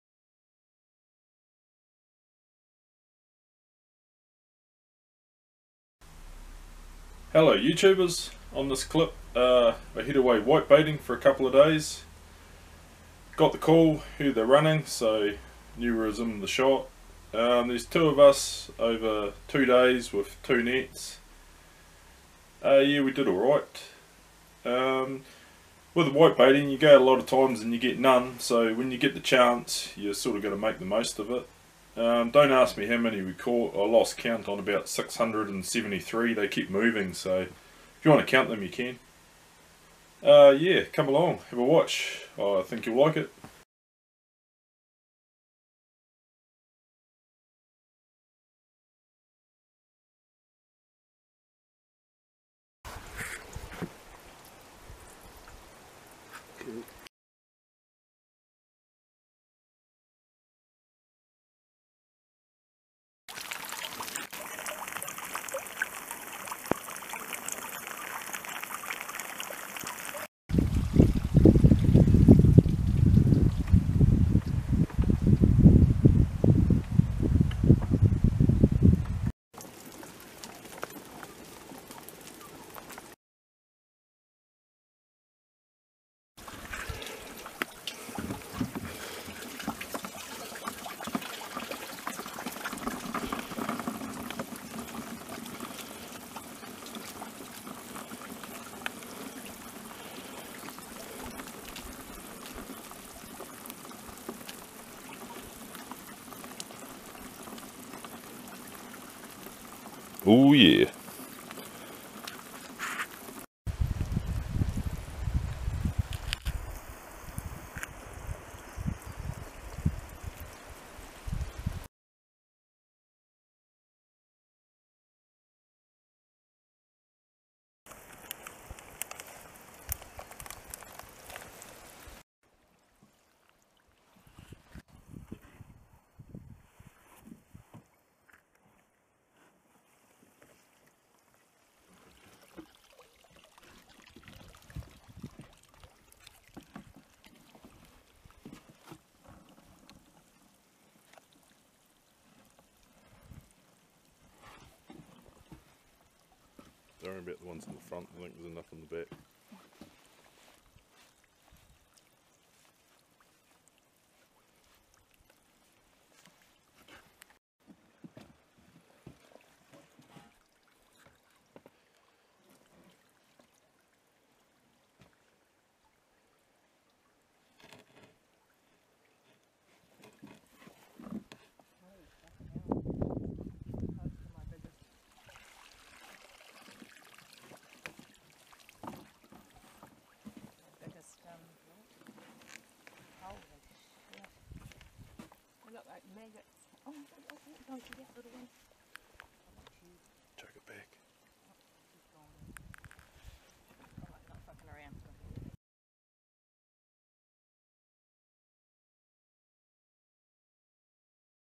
Whitebait run 2019
Highlights of two days whitebaiting while they were running.